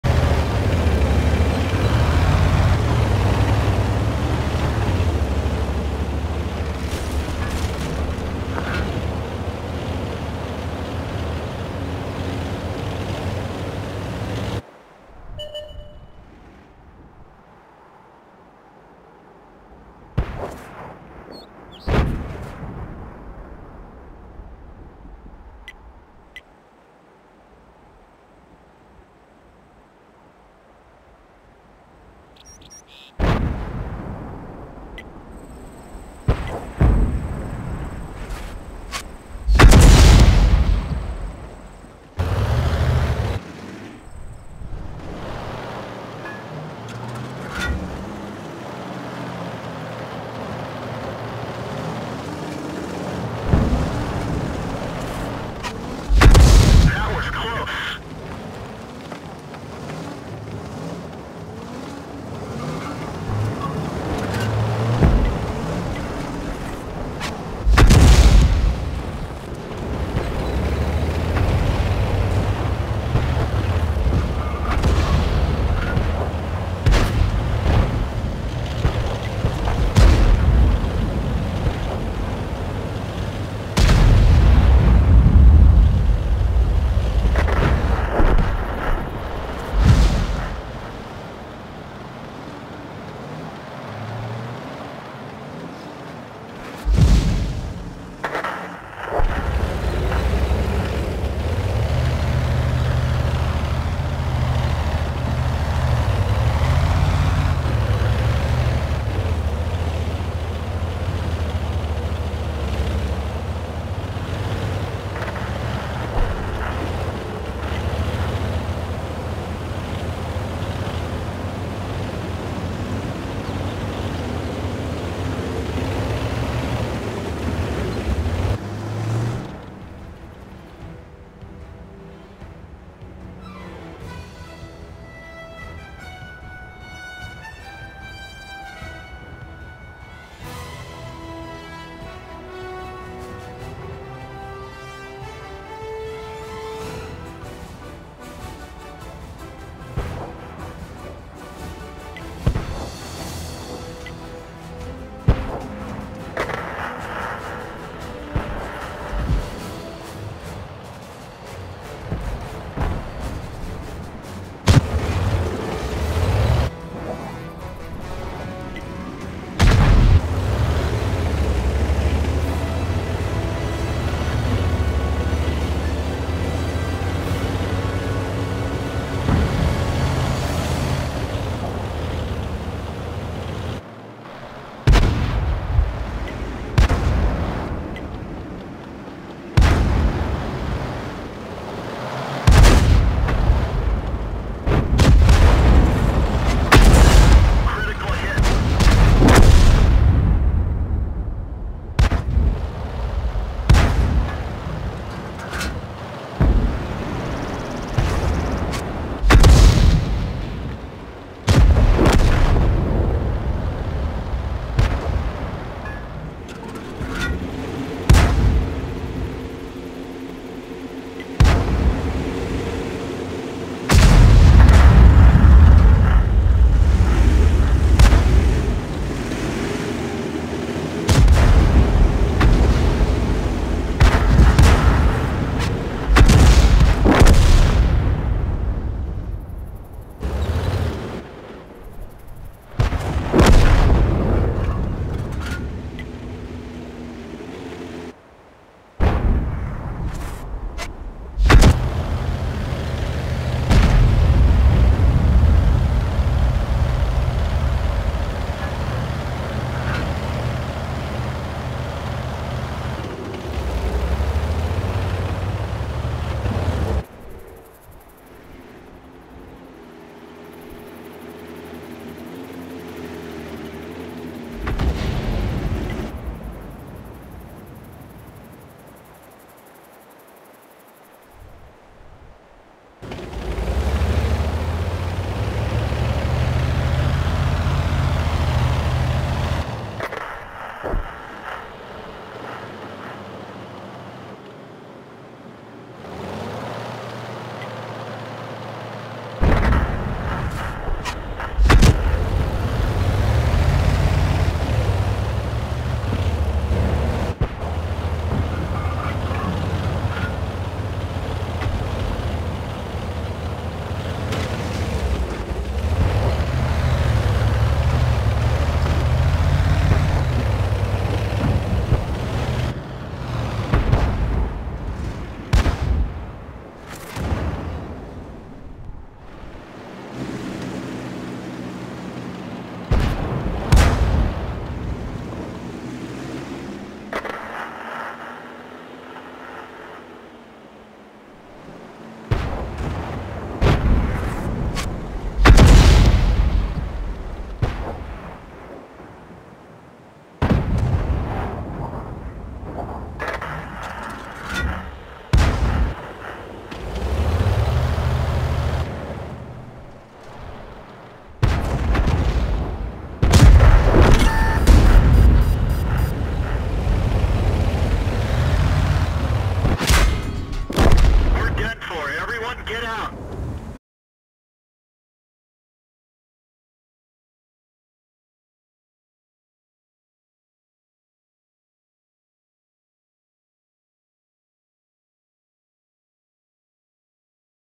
Bug, lost ally
Player: toto1904 [64PS]

TD and arty are lost at 10:15